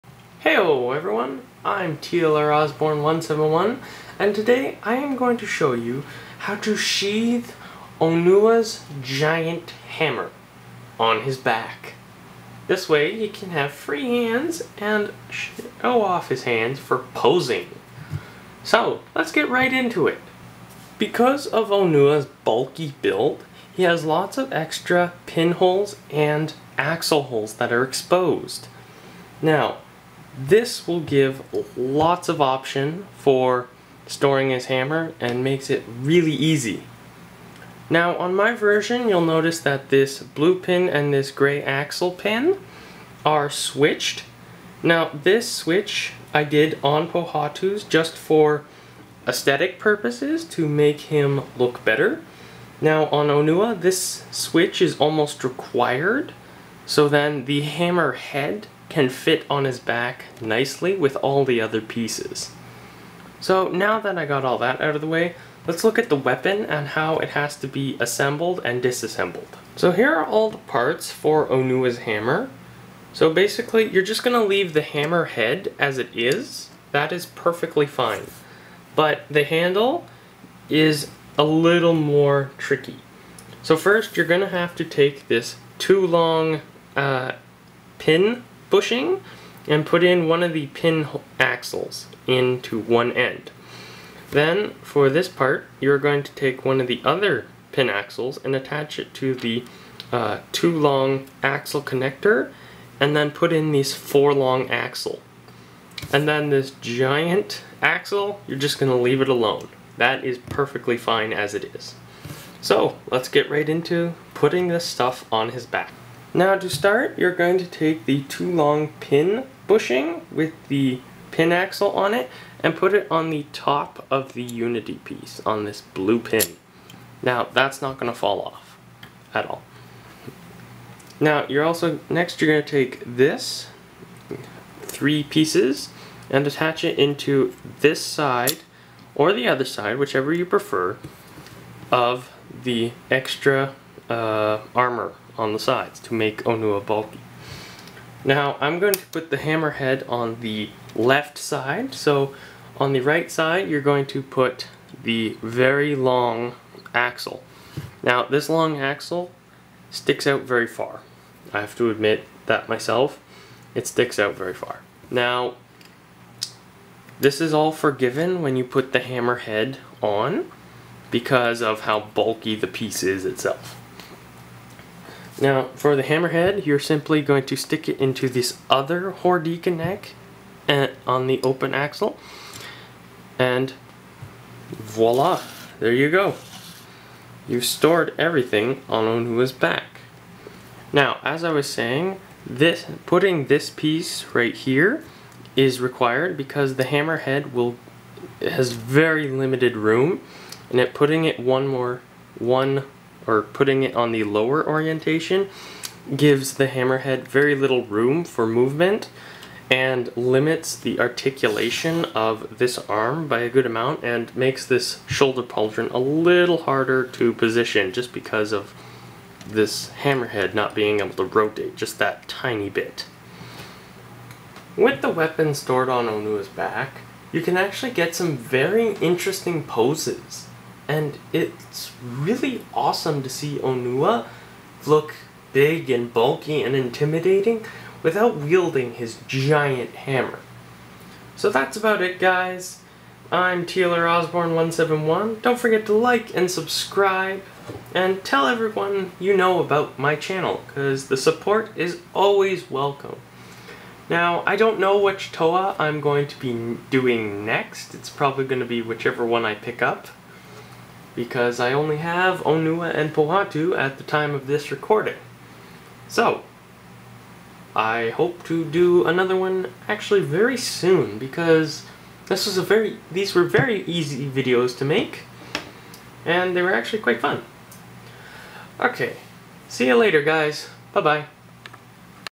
How To Sheath Onua's Hammer
In this video I show how to sheath Onua's Hammer without any extra pieces that are not part of the original set.